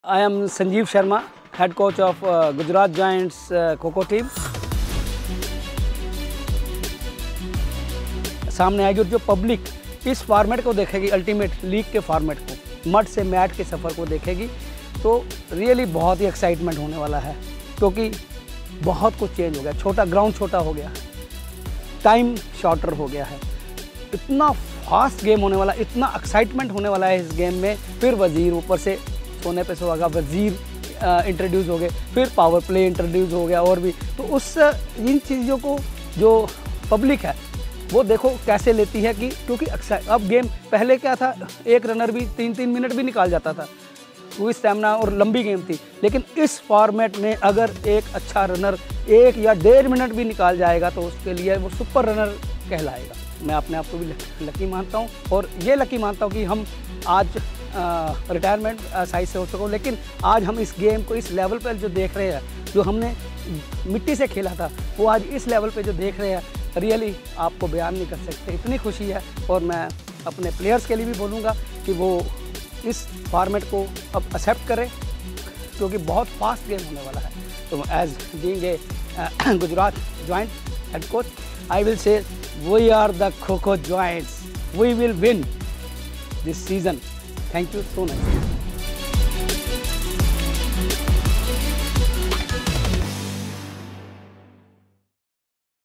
Coach Ki Zubani Episode 2 ft. Sanjeev Sharma | Ultimate Kho Kho 2022
CoachKiZubani Ep. 2⃣🎥

Is format mein agar koi runner ek ya dedh minute bhi nikaal jayega toh wo super runner kehlayega 💪

📹 | Kehna hai Gujarat Giants ke Head Coach, Sanjeev Sharma ka 🗣️